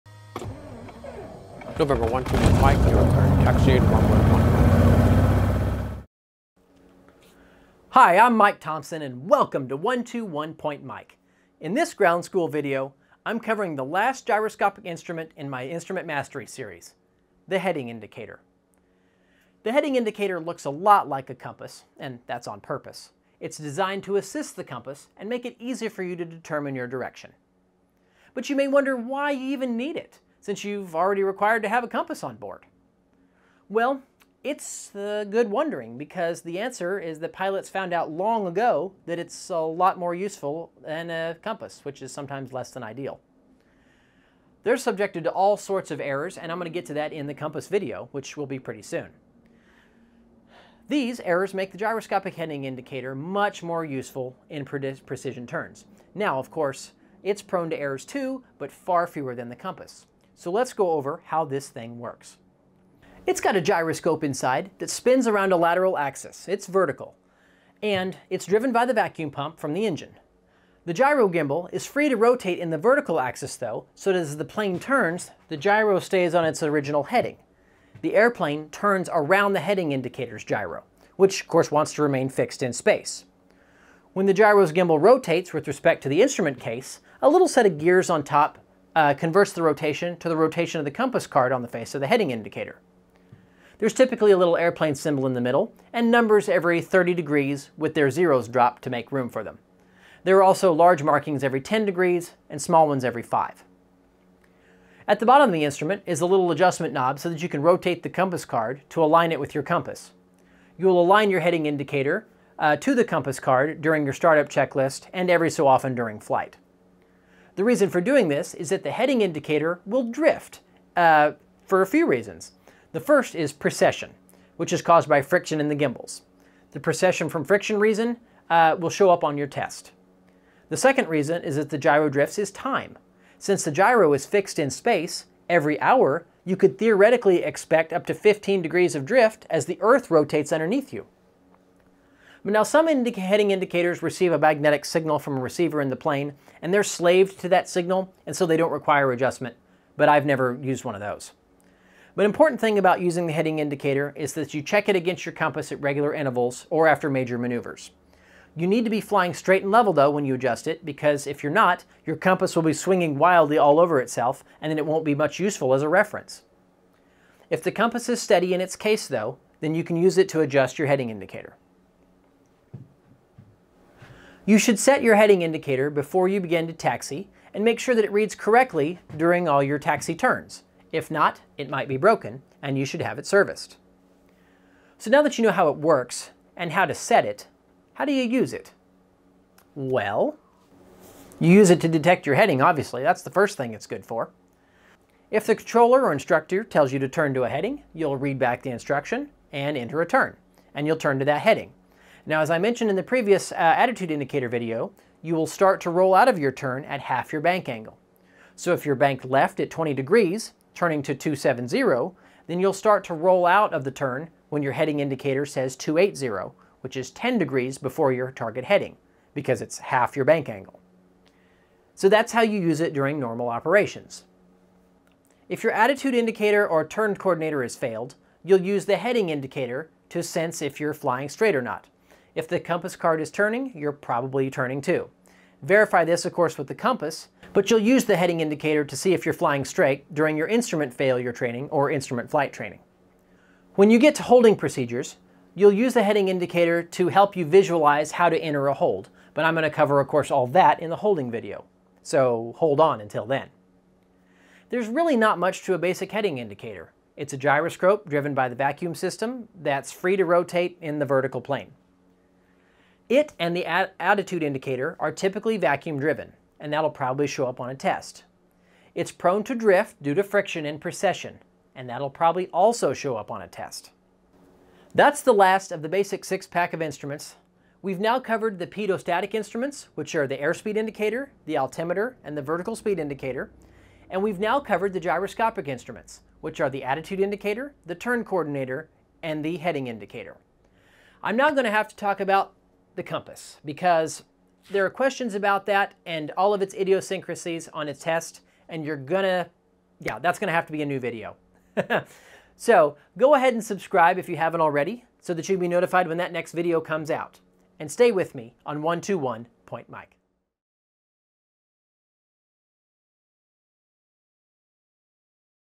HEADING INDICATOR - Instrument Mastery 7 - 121.Mike
Heading Indicators tell you which way you're going. This video tells you what you need to know for your FAA exam. I cover how they work, how to use them, errors they are prone to, and what all they are good for. Obviously, they tell you your heading and are extremely useful and less temperamental than your compass. Check out this last of the gyroscopic instruments. This video completes the basic 6-pack of flight instruments.
121.Mike is all about flying. You'll learn what you need to pass your FAA knowledge tests and how to apply this knowledge.